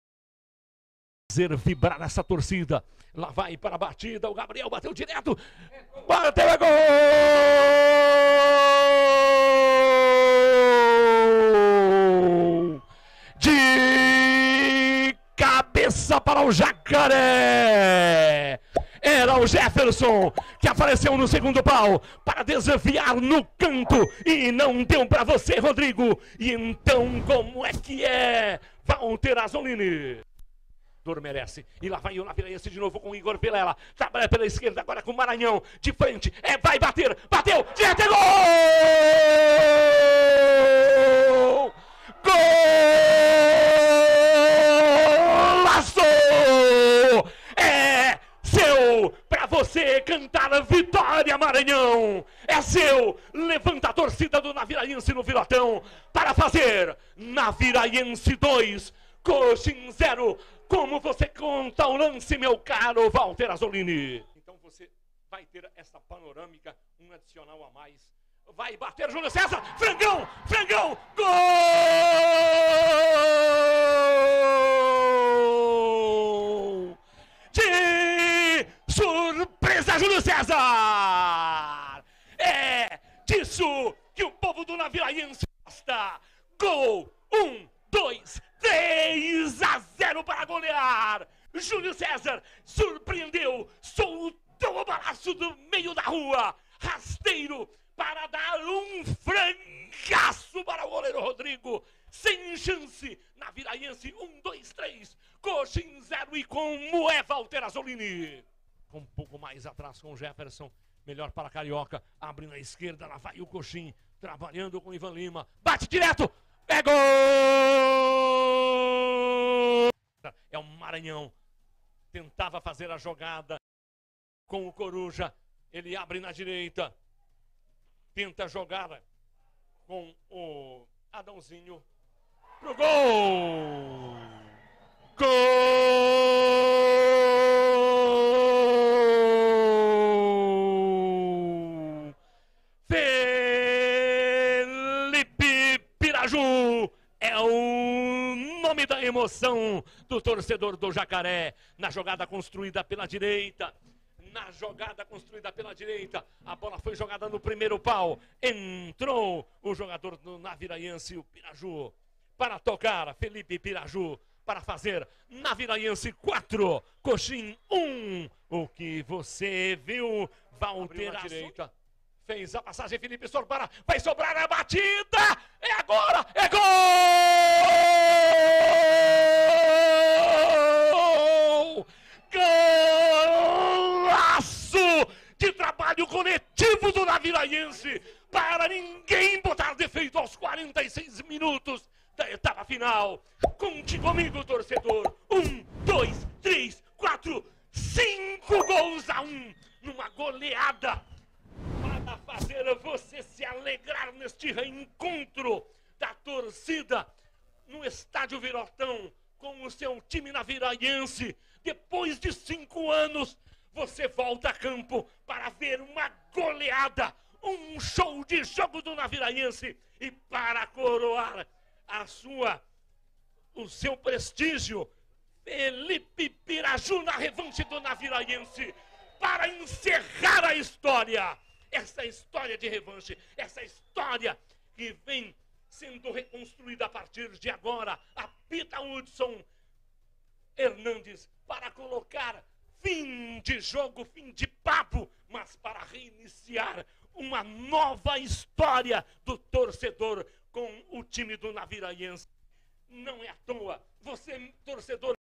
GOLS | NAVIRAIENSE 5 x 1 COXIM | CAMPEONATO SUL-MATO-GROSSENSE 2022
CAMPEONATO SUL-MATO-GROSSENSE 2022

NAVIRAIENSE 5 x 1 COXIM

ESTÁDIO VIROTÃO